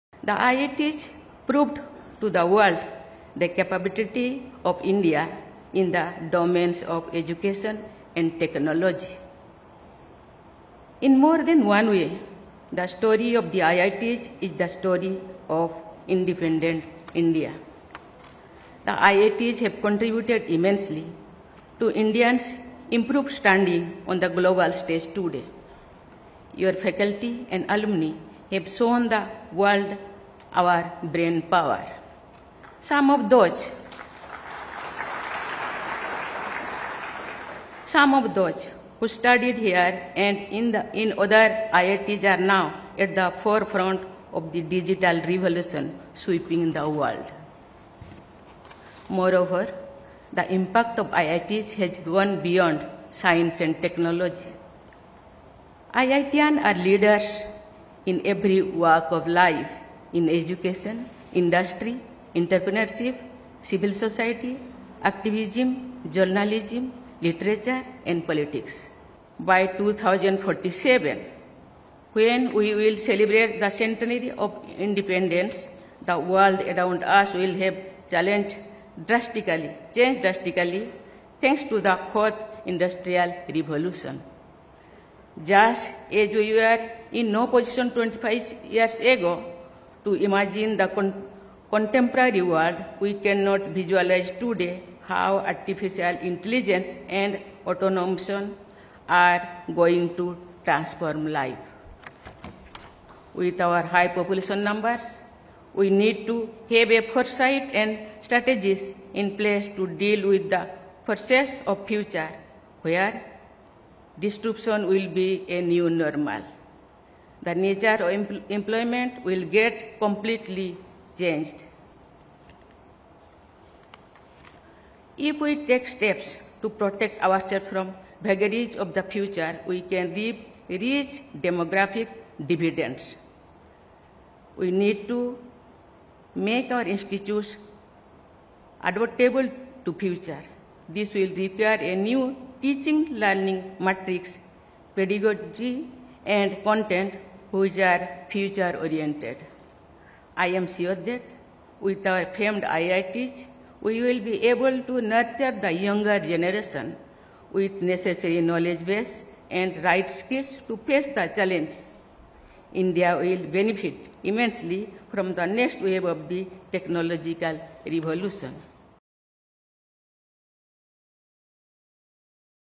President Droupadi Murmu: "Institutes should adapt to new-age technologies"
Highlighting the evolving technologies and changing employment dynamics, President Droupadi Murmu called upon educational institutes to become adaptable to the future. Speaking at the Diamond Jubilee celebrations of IIT Delhi today, President Murmu said, "IITs proved to the world the capability of India in the domains of education and technology.